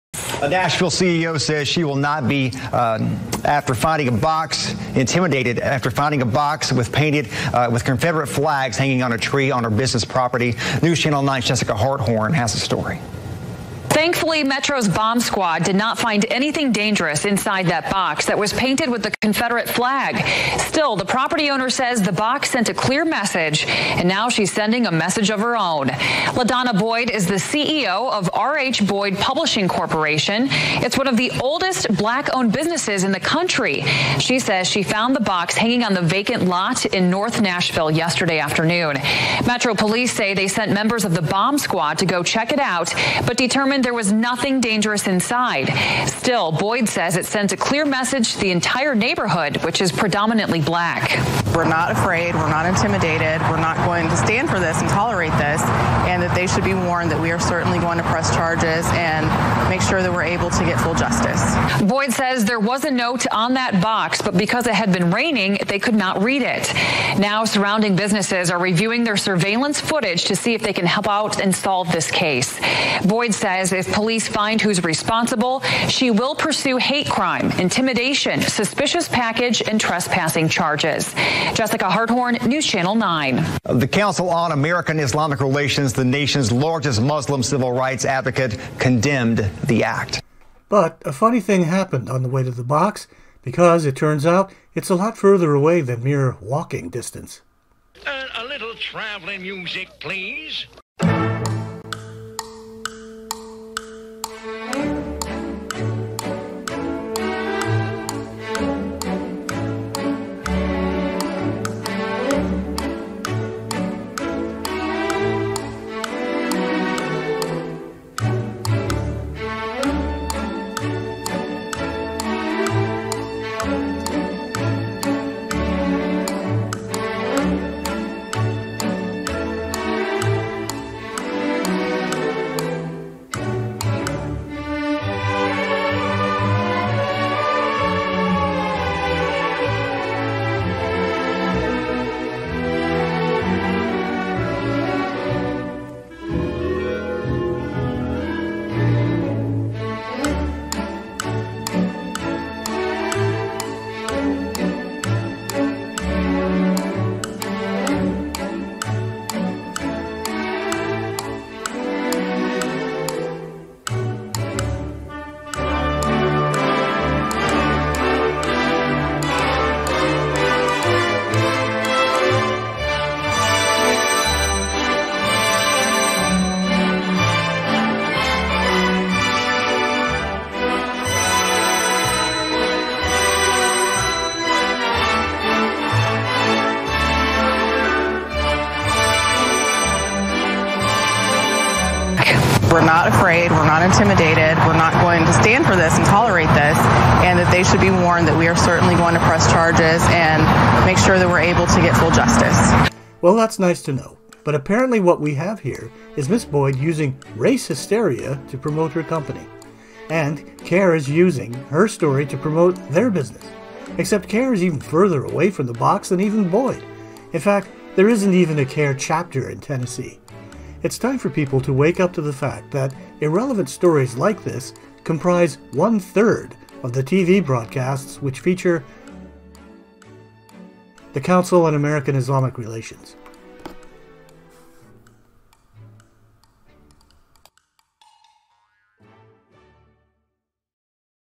CAIR and the Confederate Box
The media, the business owner, and CAIR all want a piece of the Confederate Box.